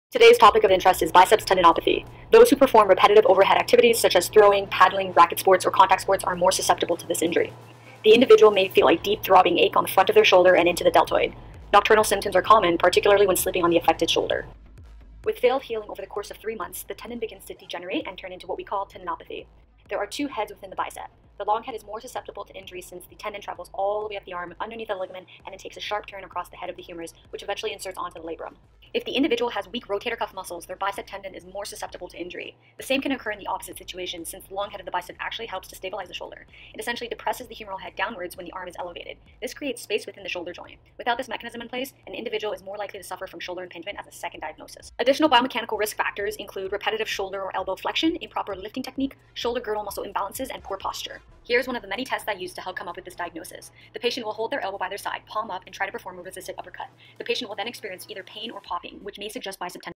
Bicep Tendinopathy| Tendinitis | Tendinosis
💪🏼Today’s topic of interest is biceps tendinopathy.

🏸Those who perform repetitive overhead activities such as throwing, paddling, racquet sports, or contact sports are more susceptible to this injury.

💢The individual may feel a deep throbbing ache on the front of their shoulder and into the deltoid.

💤 Nocturnal symptoms are common, particularly when sleeping on the affected shoulder.

🔬 Chronic overload of the tendon leads to microscopic tears. With failed healing over the course of 3 months, the tendon begins to degenerate and turns into what we call “ tendinopathy”

✌🏼 There are two heads within the bicep. The long head is more susceptible to injury since the tendon travels all the way up the arm underneath a ligament and then takes a sharp turn across the head of the humerus, which eventually inserts onto the labrum.

😩If the individual has weak rotator cuff muscles, their bicep tendon is more susceptible to injury.

🤓The same can occur in opposite situation since the long tendon of the bicep actually helps to stabilize the shoulder by depressing the humeral head downwards when the arm is elevated. This creates space within the shoulder joint. Without this mechanism in place, an individual is more likely to suffer from shoulder impingement as a second diagnosis.

🏋🏽‍♂️Additional biomechanical risk factors for the development of biceps tendinopathy include repetitive shoulder or elbow flexion, improper lifting technique, shoulder girdle muscle imbalances and poor posture.

✔️Here’s one of the many tests I use to diagnosis this. The patient holds their elbow by their side, palm up, and tries to perform a resisted upper cut. The patient will experience pain or popping, which suggests bicep tendon involvement